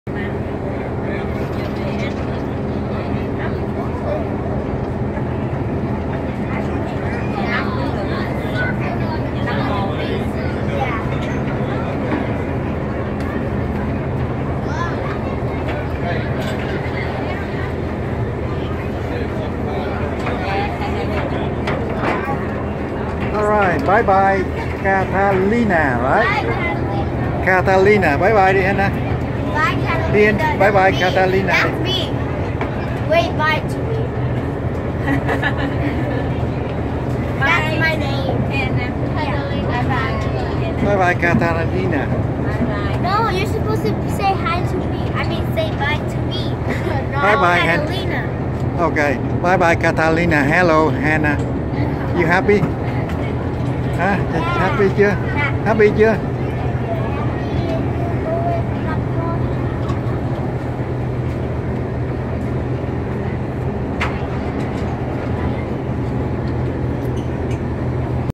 Back to the Carnival Inspiration Cruise Ship from Catalina Island by a small boat - Family Vacation.
Back to the Carnival Inspiration Cruise from Catalina Island by a small boat - Family Vacation.